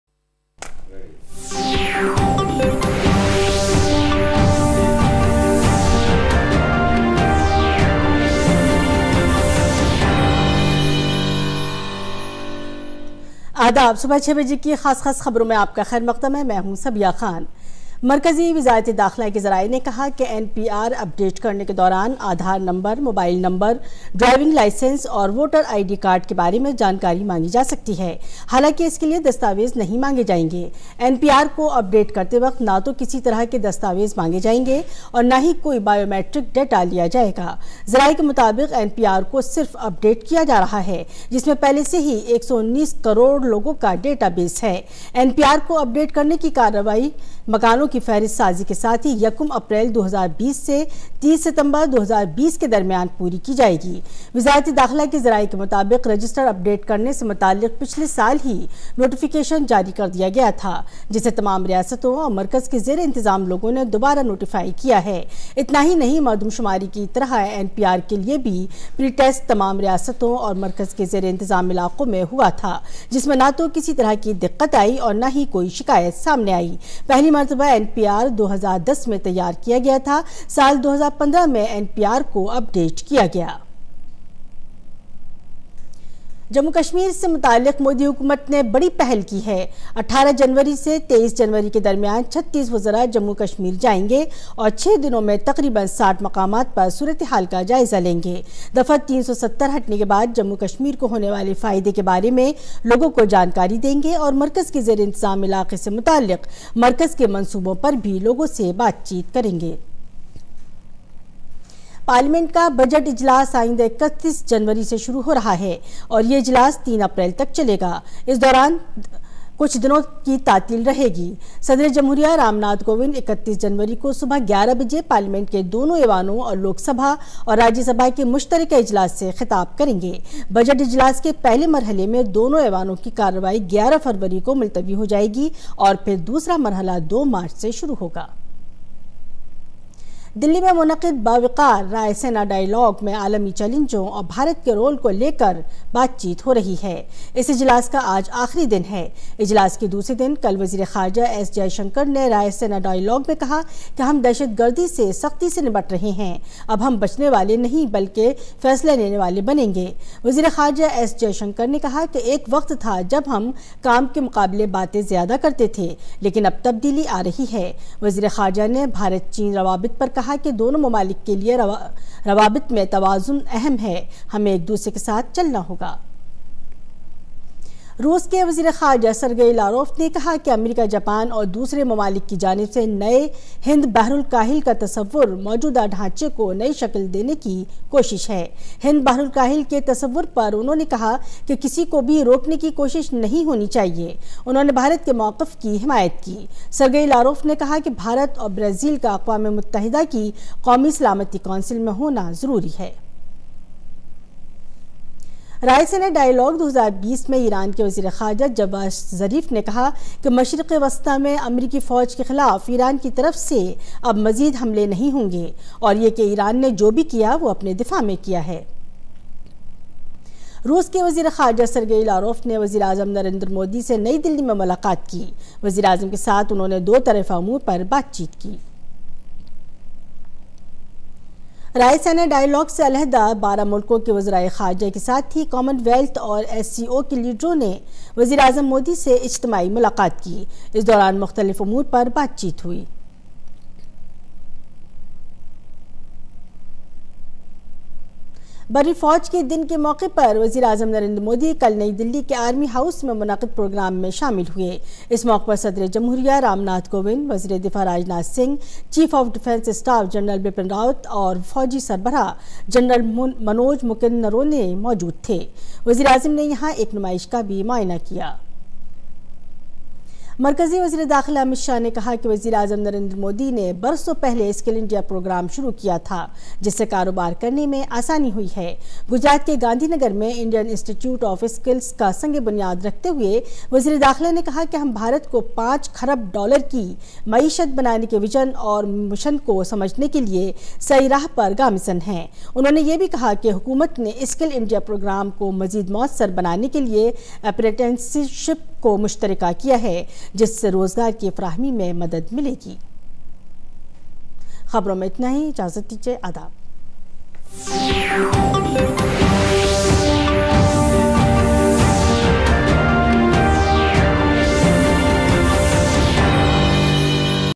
Urdu News No Papers will be asked and No biometrics will be taken
Enjoy our 6 am Urdu News Bulletin daily and get yourself updated with Major News of the day | 16 January, 2020